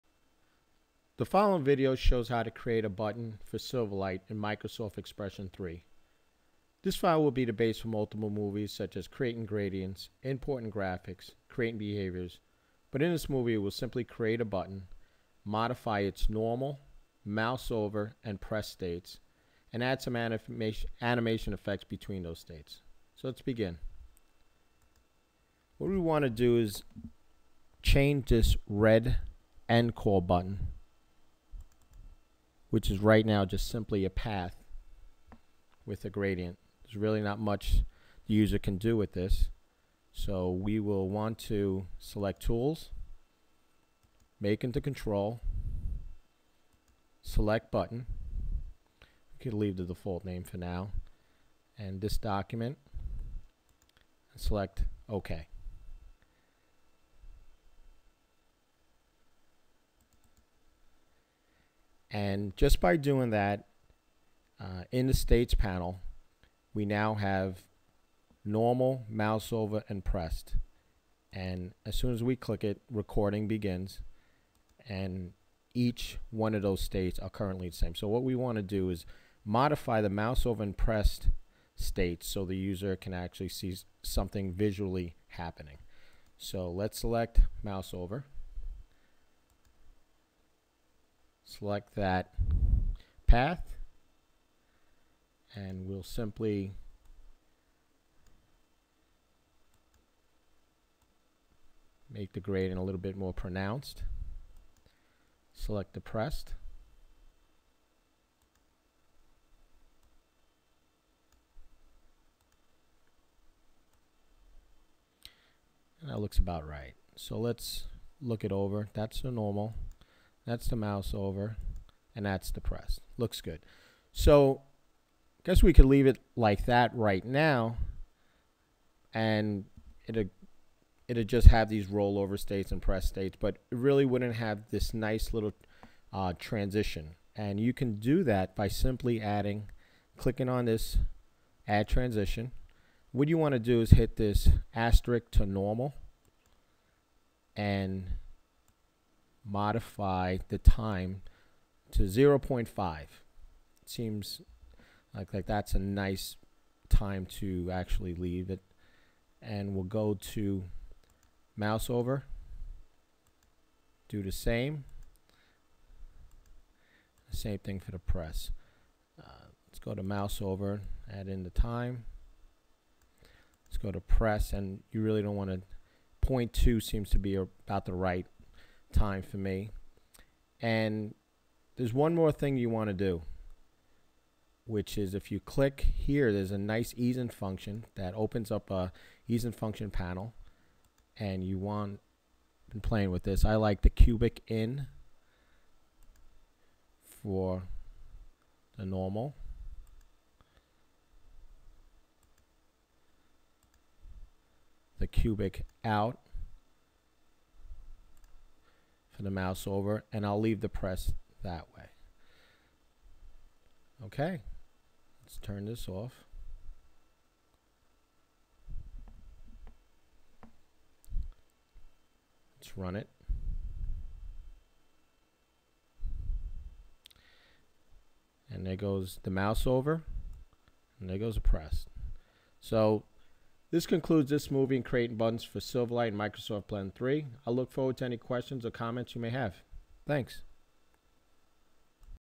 SilverLight 3 - Creating a button with Microsoft Blend 3
This video tutorial will show how to create a custom button, modify its normal, mouse over and pressed states. Also add animation effects between those states.